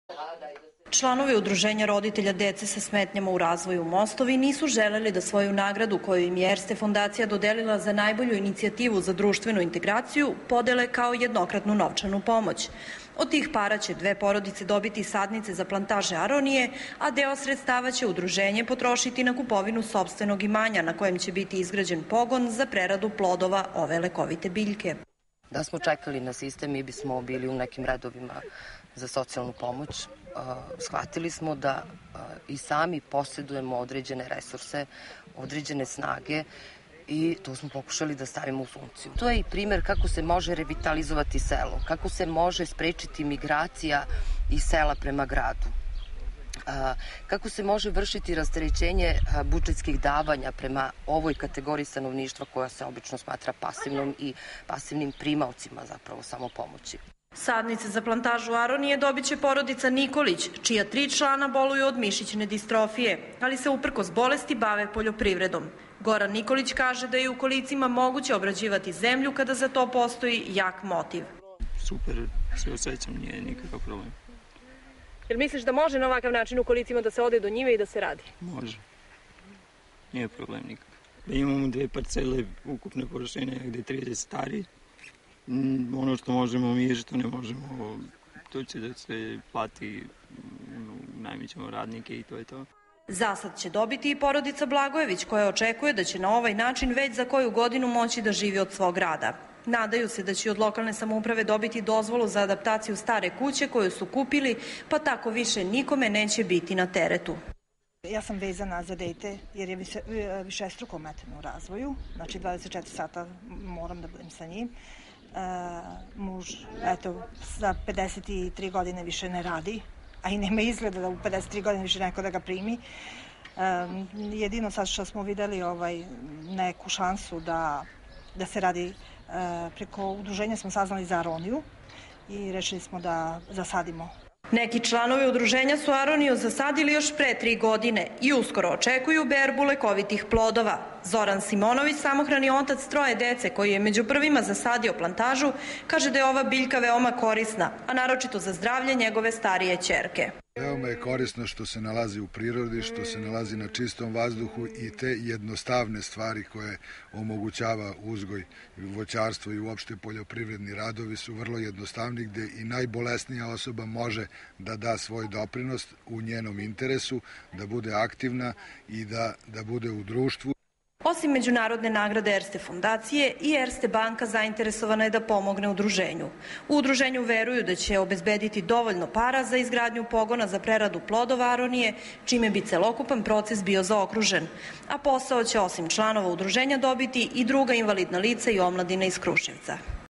Neće socijalnu pomoć, sade aroniju
Umesto da čekaju u redovima za socijalnu pomoć roditelji dece sa smetnjama
u razvoju iz Kruševca odlučili su da zasade plantaže lekovite biljke
aronije. Udruženje Mostovi će svoju nagradu od 16.000 evra, koju je dobilo
od Erste fondacije, potrošiti za kupovinu sadnica i izgradnju pogona za
preradu plodova aronije.